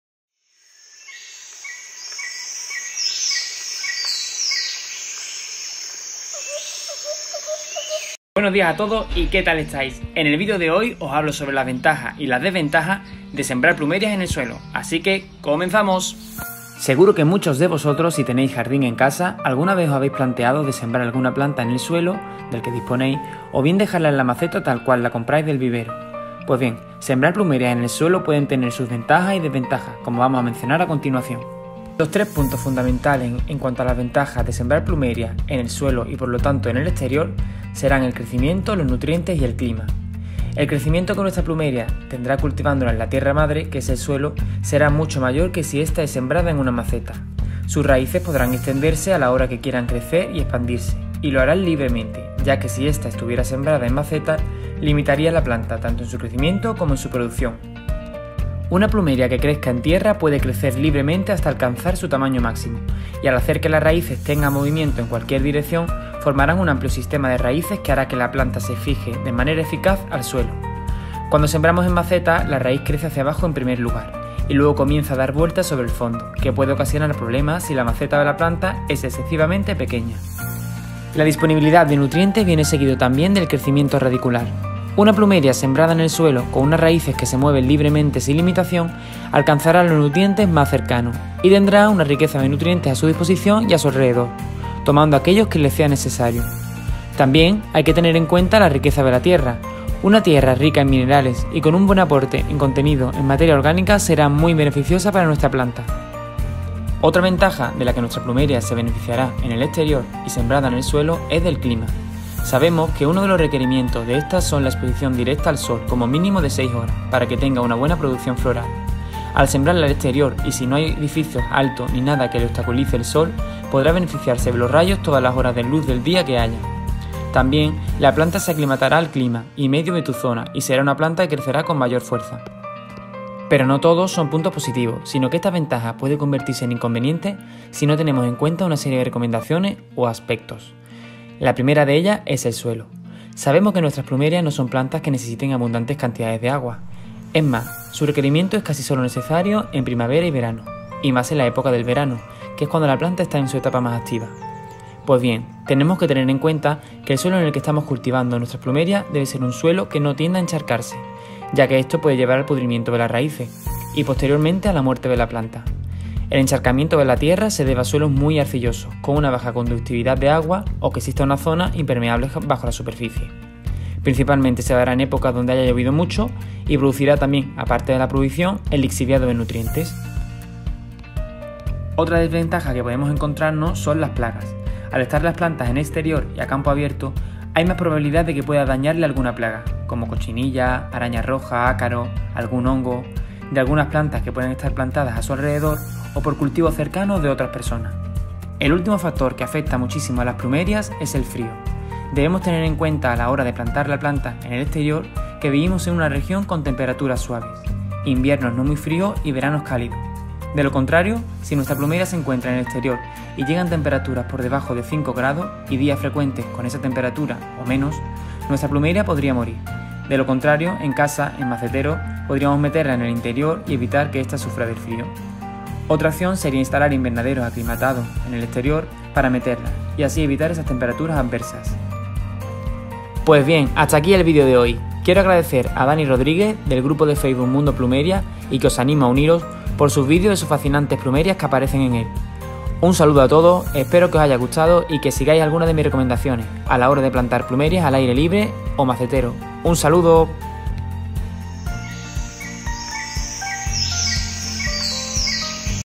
😍🌺Cultivando PLUMERIAS en el exterior, ¿ Qué debes saber antes?. VENTAJAS E ICONVENIENTES 🤔 🌱
En este nuevo video te cuento las ventajas e inconvenientes de cultivar plumerias en el suelo.